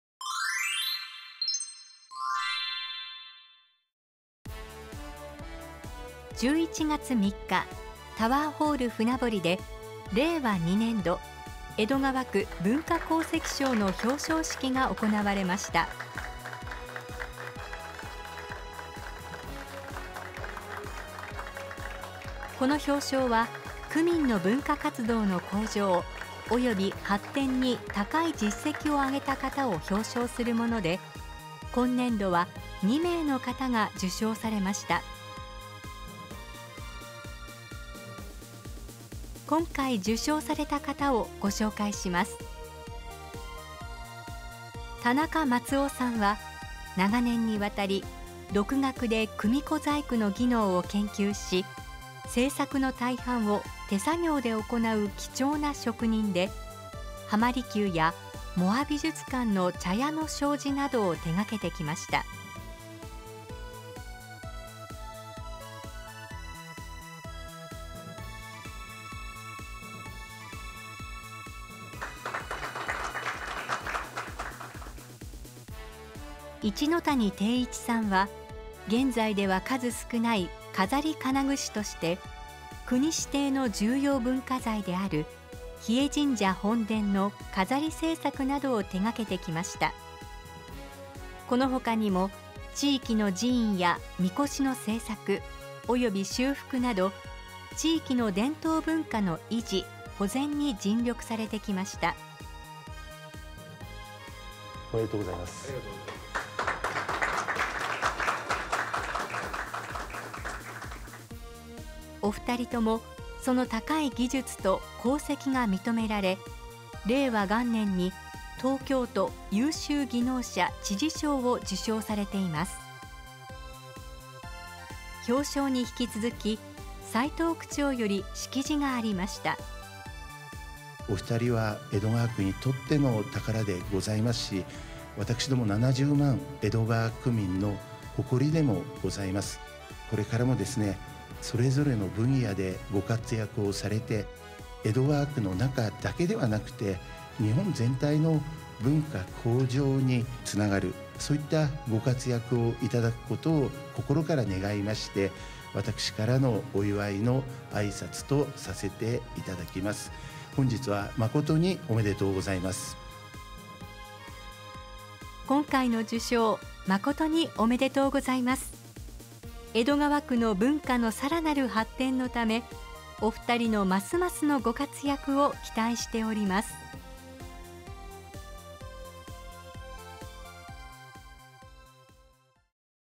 令和２年度 江戸川区文化功績賞 表彰式
この表彰は、区民の文化活動の向上および発展に高い実績をあげられた方をたたえ、表彰するものです。今年度は、組子建具の分野で活躍されている田中 松夫さんと錺金具の分野で活躍されている一ノ谷　禎一さんの２名の方が文化功績賞に受賞されました。その様子をご覧ください。